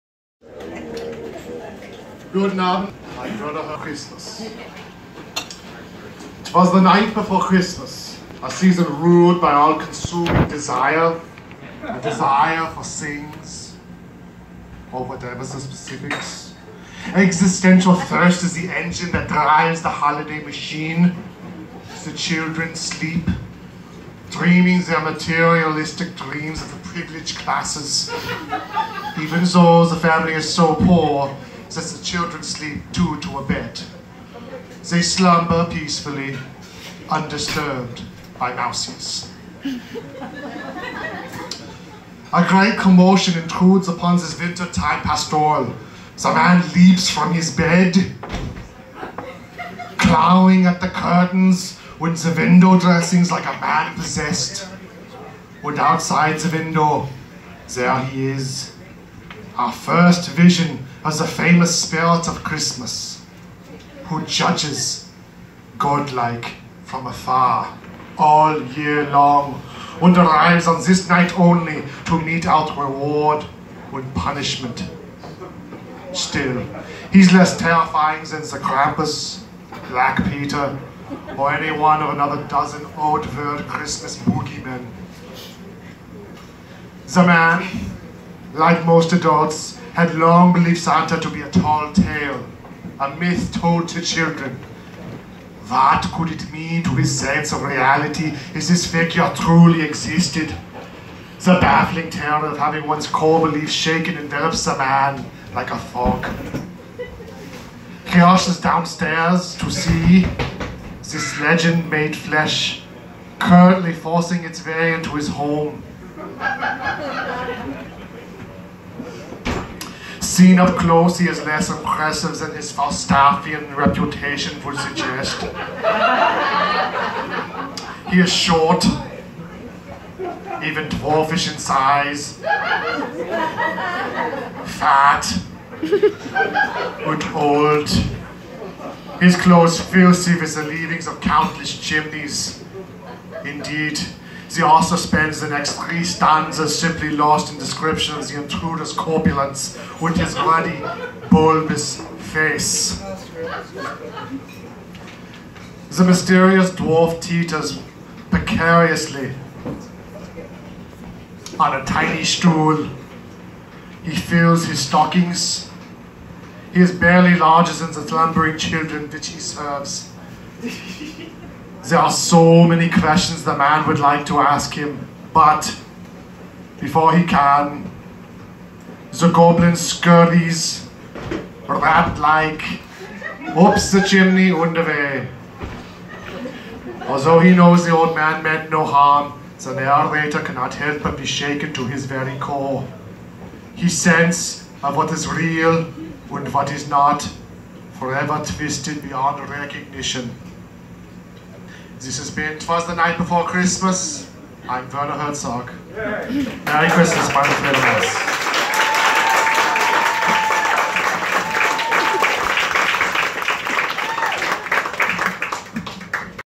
MCCT Performance of Twas The Night Before Christmas
Me as Werner Herzog interpretating Twas The Night Before Christmas